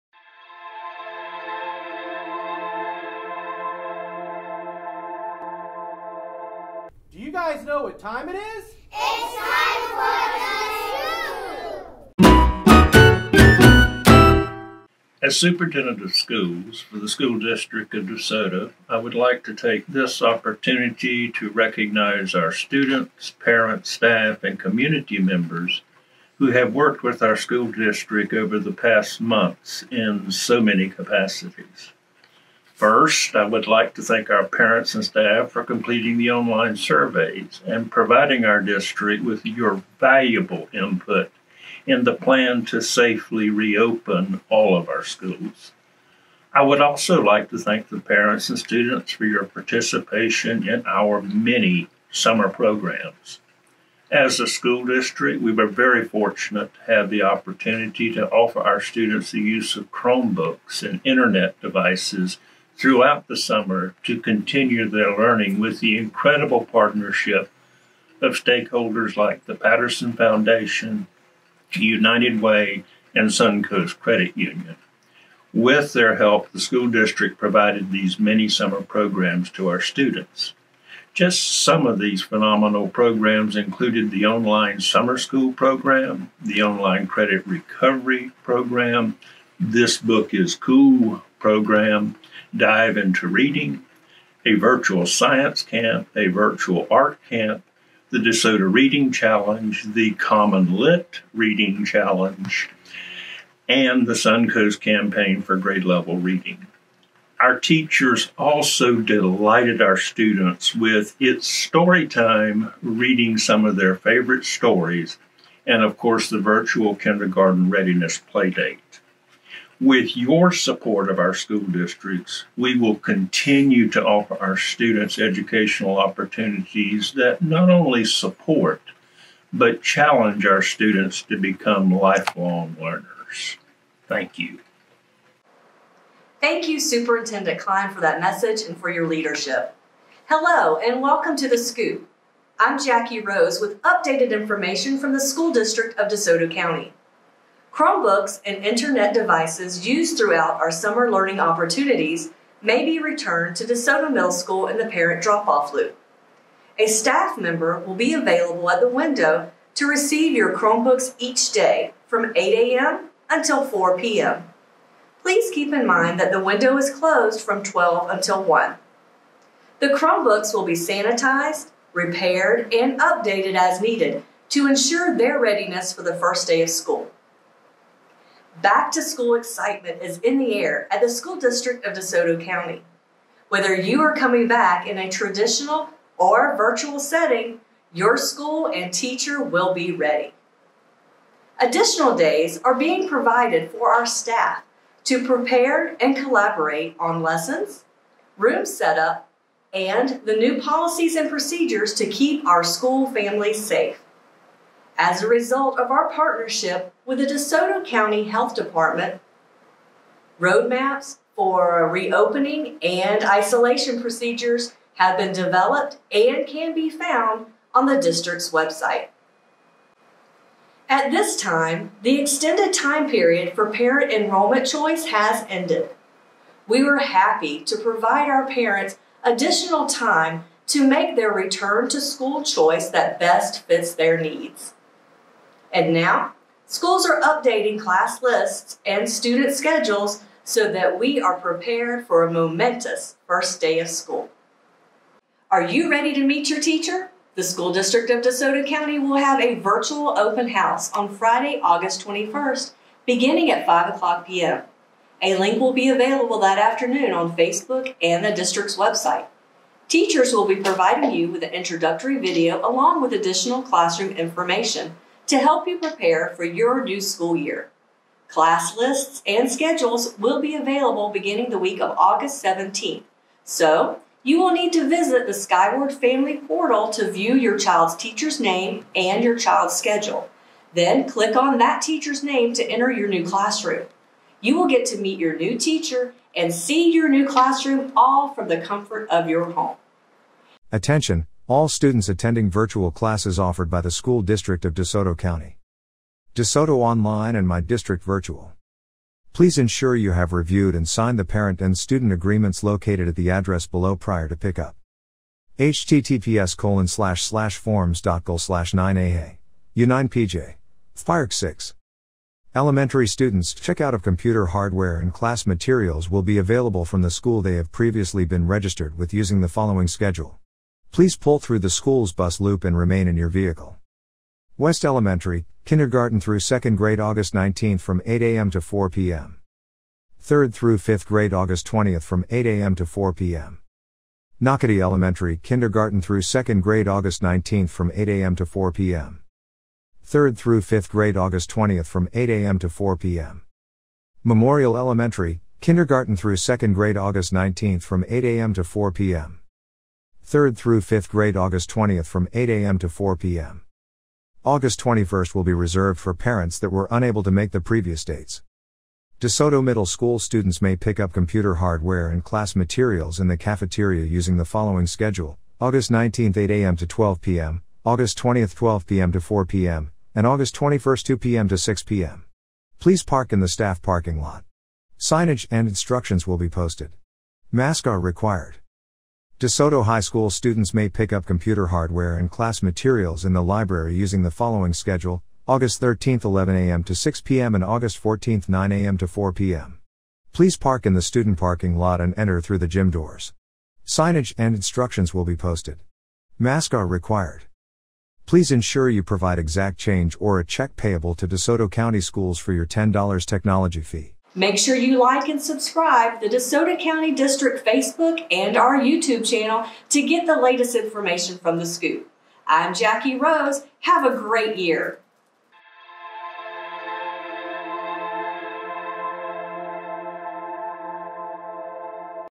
Back to School Information from The School District of DeSoto County
Jackie Rose is back with important information about the upcoming school year from Chromebook Returns, Virtual Open House, and so much more. She is also joined by our Superintendent who has an important message for parents and students. Do not miss the first Scoop of the new school year!

Please ensure you have reviewed and signed the “Parent and Student Agreements” located at the address below prior to pick-up: